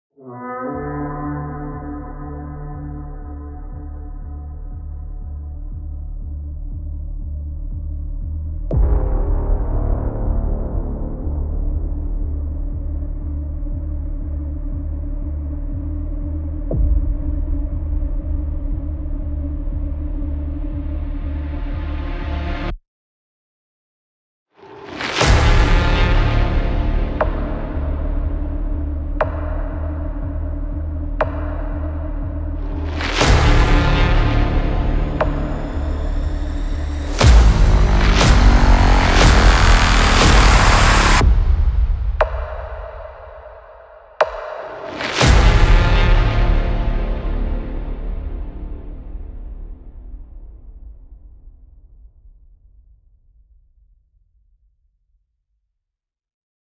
AERA | The Core Of Trailer SFX | FREE Cinematic Trailer Sound Effects | SUBMORITY
AERA is a FREE library with 20 hard-hitting and cutting-edge sound effects for trailers as well as for movies, television, video games, and other visuals. This small but complex package contains a wide spectrum of sounds that are a solid foundation for transforming your every project into a real cinematic beast.

The idea behind this library was to create a modern toolkit that is the essence of trailer sound effects. With one sound per category, AERA is the perfect starting point for music composers and editors in need of adding a punch to their projects in a flexible way.

Ⓒ Submority